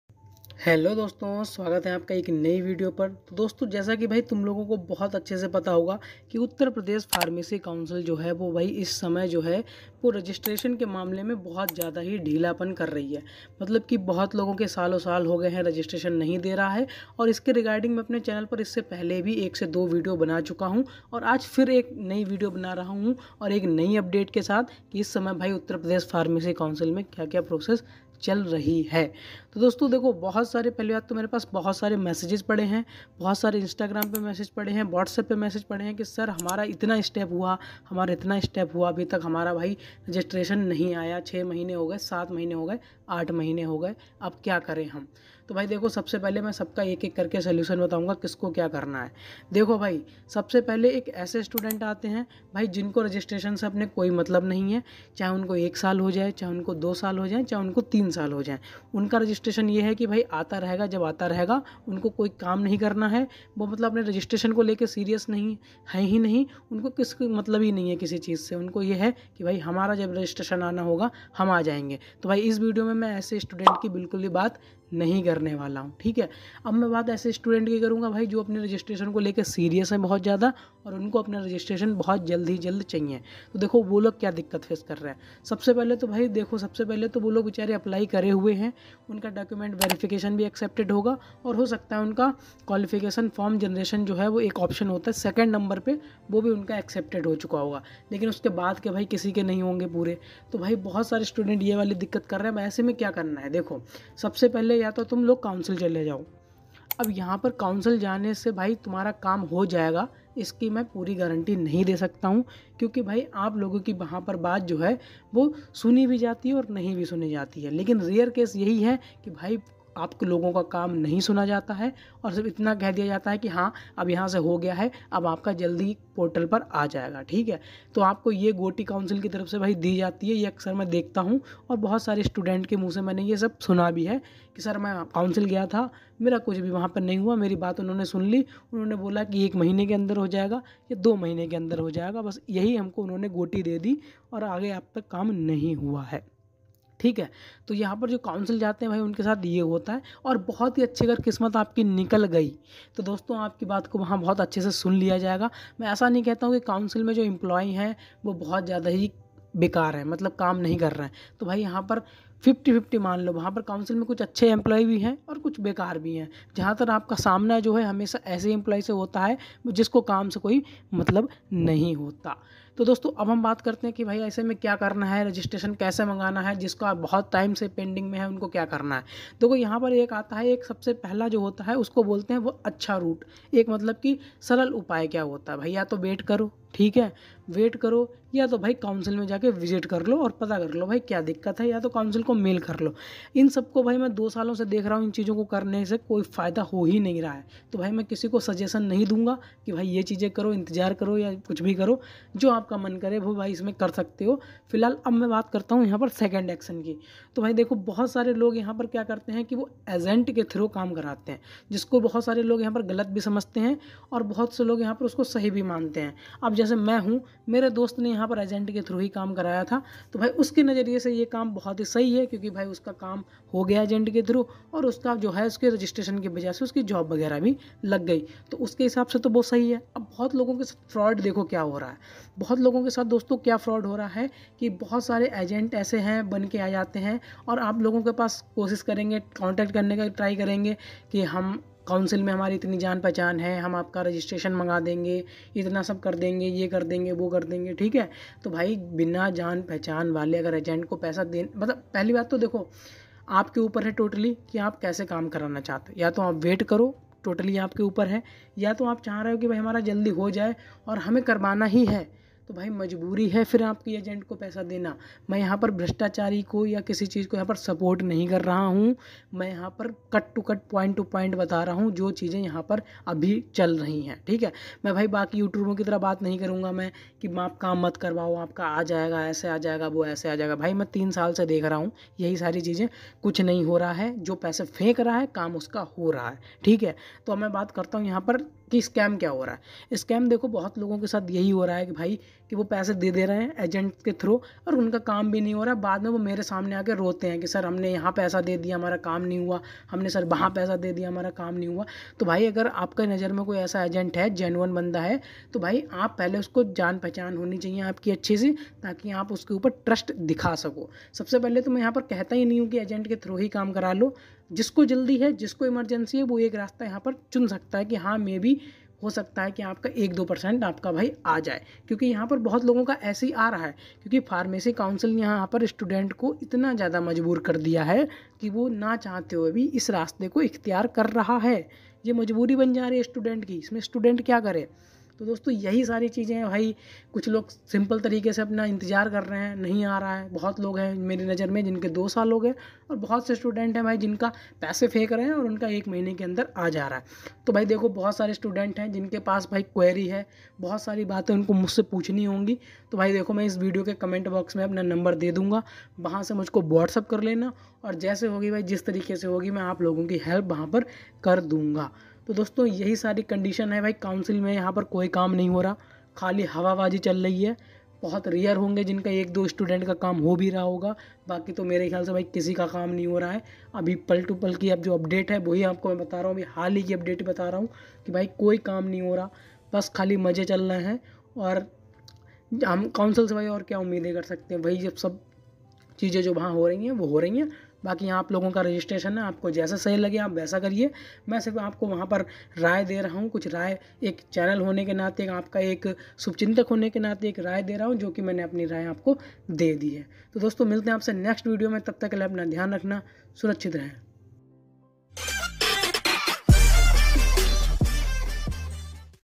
UP Pharmacy Council Online Registration || कब तक आयेगें Pending में पड़े हुए Registration
👉This is Akshay dosto is video Me mne apko UP Pharmacy Council se Related Problems Ke bare me Bataya hain.

👉 Boya mic
👉 Green Screen